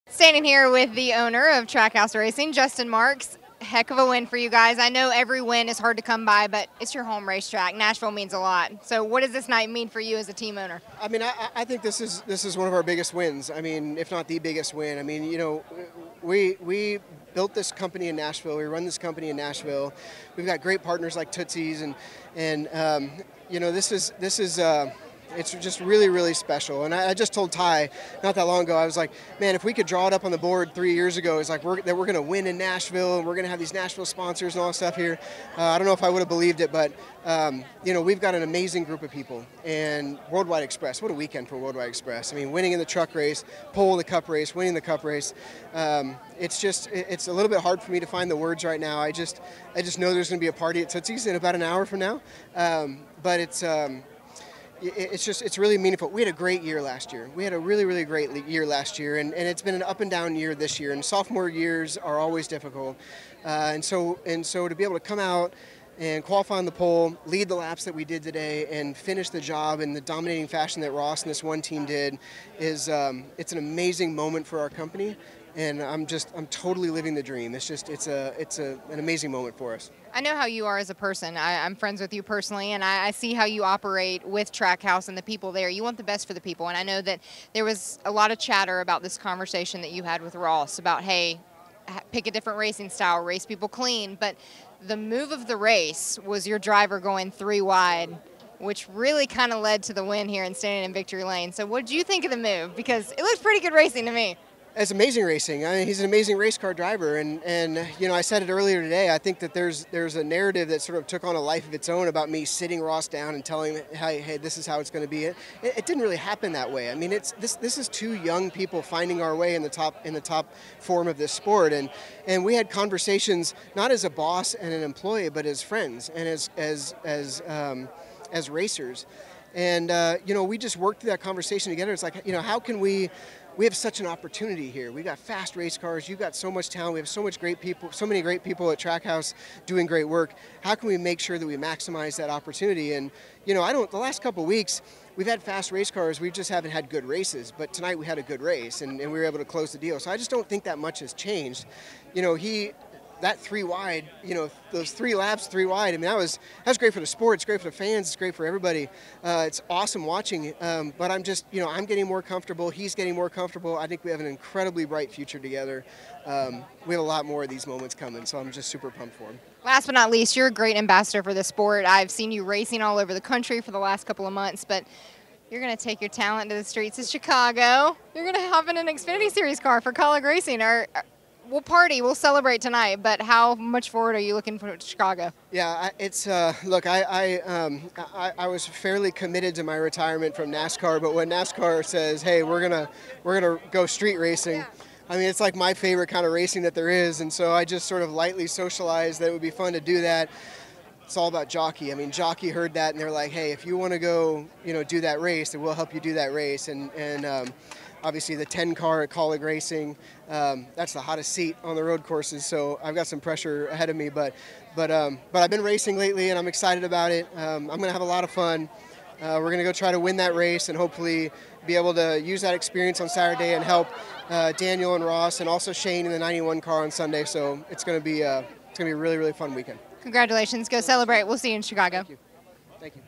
Justin Marks: Nashville win is 'amazing moment' for Trackhouse
Justin Marks calls Ross Chastain's win at Nashville an 'amazing moment' for Trackhouse Racing after winning the pole and the race.

Play every weekend!